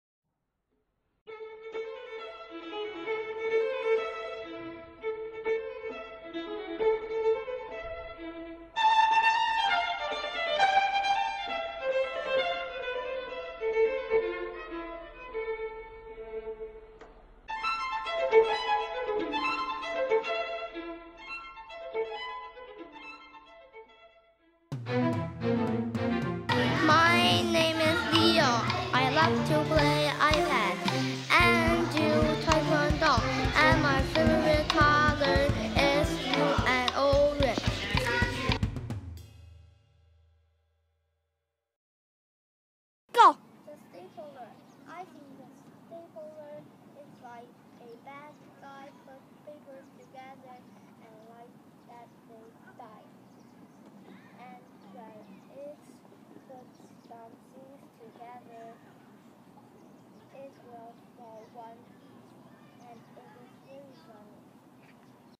Leo
During the IT class, students created a poetry presentation on the PowerPoint. They typed different poems using a variety of styles and at the same time, they searched, copied and pasted the animated (GIF) images from the internet onto their slides. They were excited to see the GIF images and shared them with other students.
To make the presentation more interactive, we went outside to the playground and the students chose a spot to record their poems while making videos of each other using the iPads. They explored and learned many of the videoing techniques. Students inserted their videos. They also added their raps which were created and recorded in the music class with Ms. Jan and Ms. Melita.  They made beautiful cover pages for their presentations during the art class with Ms. Hart and Ms. Jensen. Finally, they added the transitions and music on their slides.